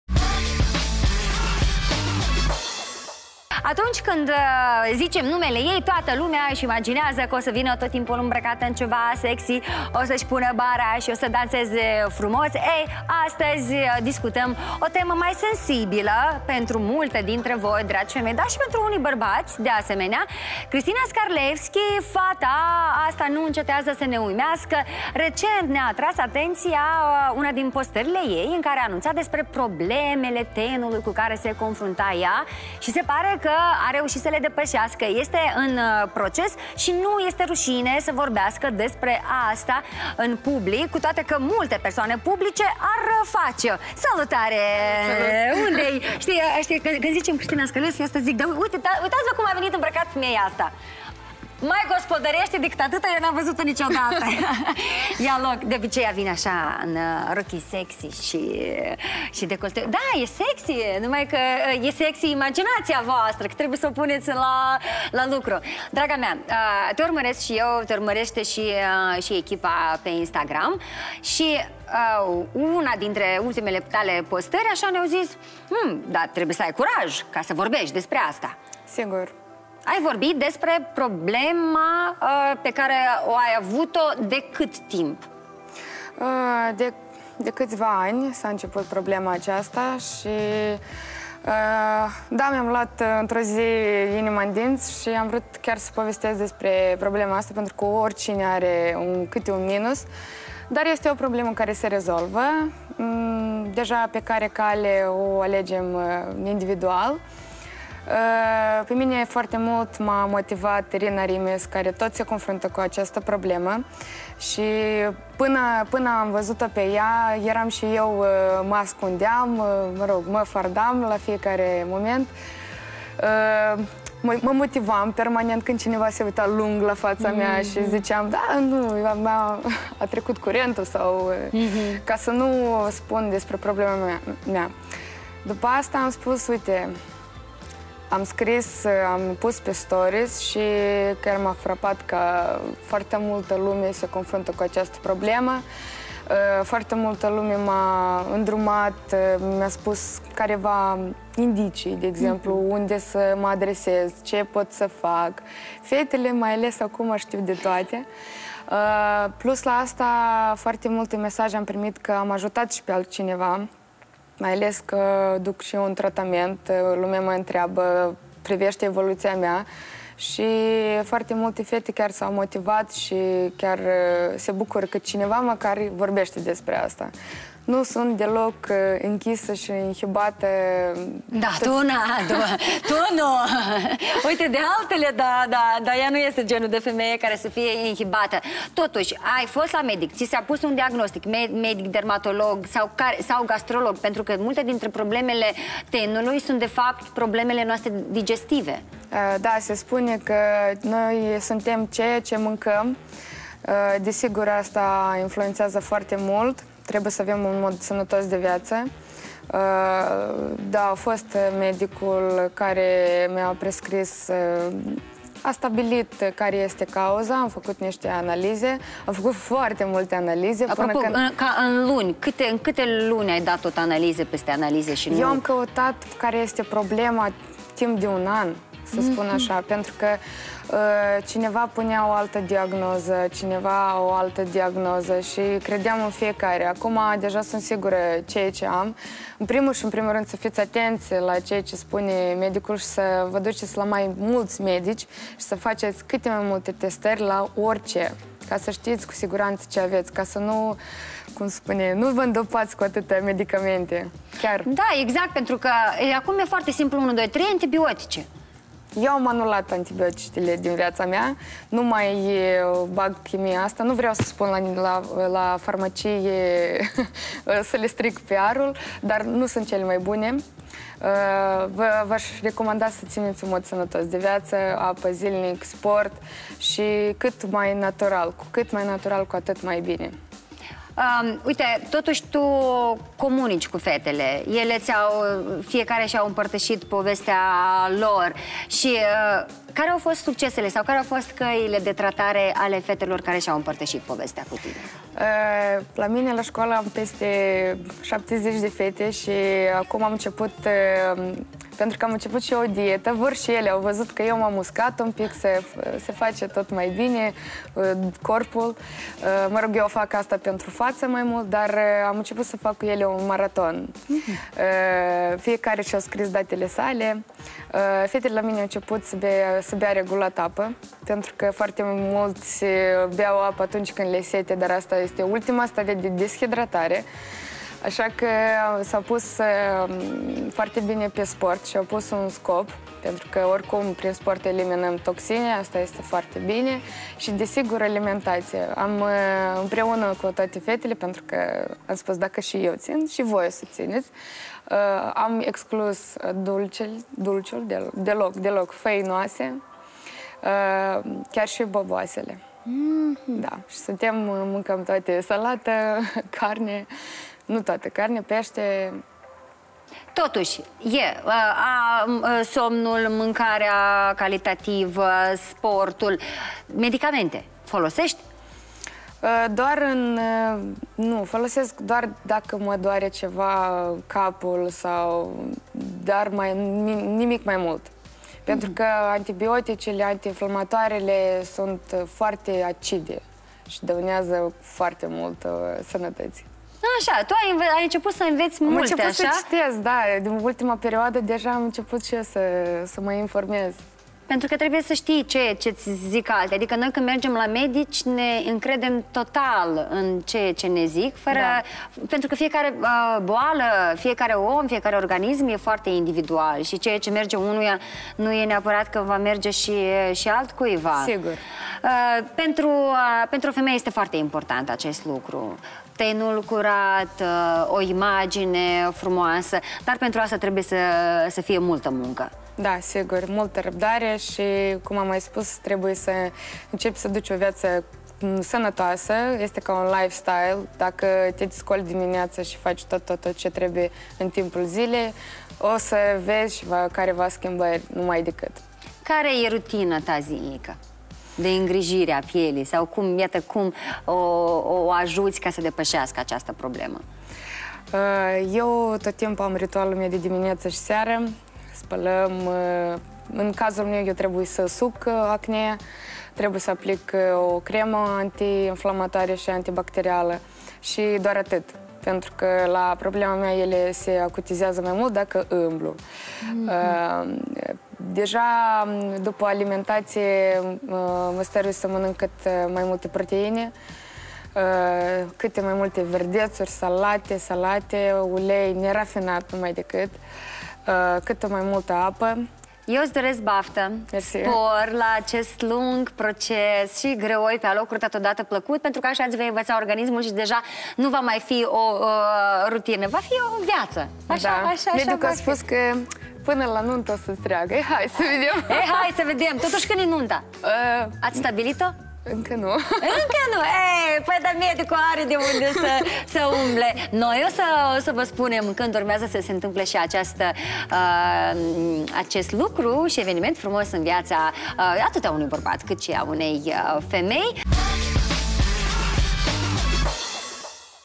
Află cum îngrijești tenul acneic. Destăinuiri cu CRISTINA SCARLEVSCHI
A fost complexată din cauza acneei! Cristina Scarlevschi de mai mulţi ani se confruntă cu aceasta problemă a tenului! Vedeţi cum îşi ingrijeşte acum tenul!